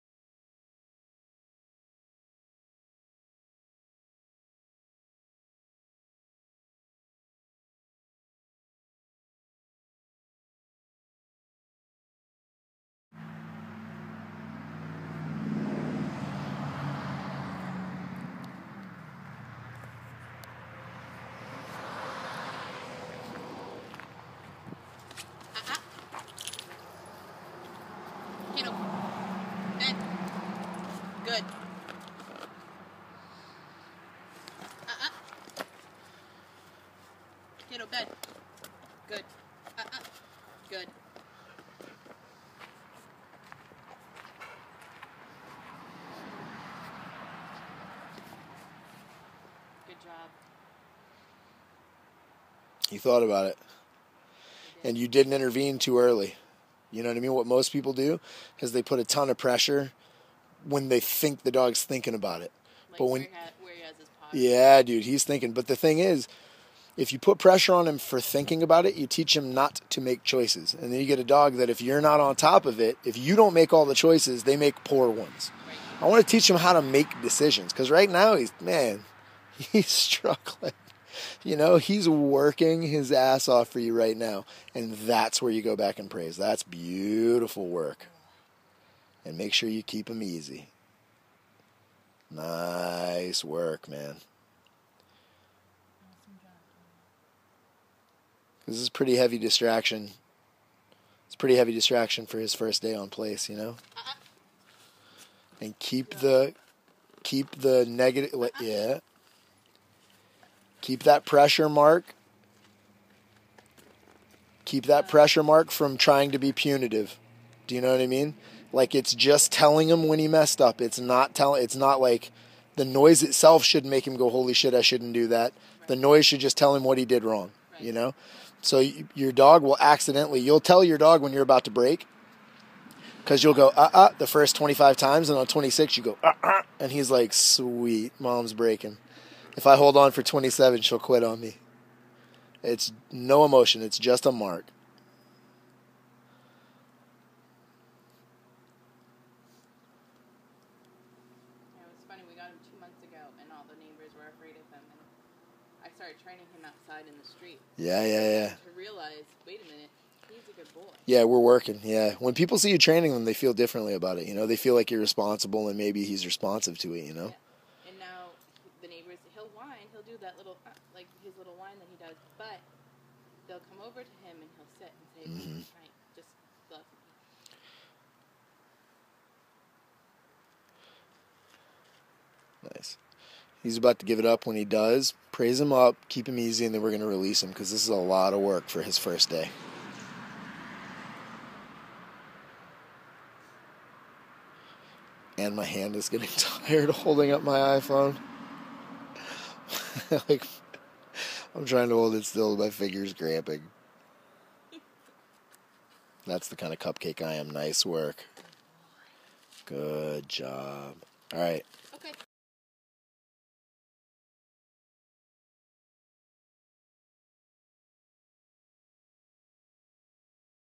Kato's Progress
Here's a little uplifting story of a dog taking advantage of his second chance.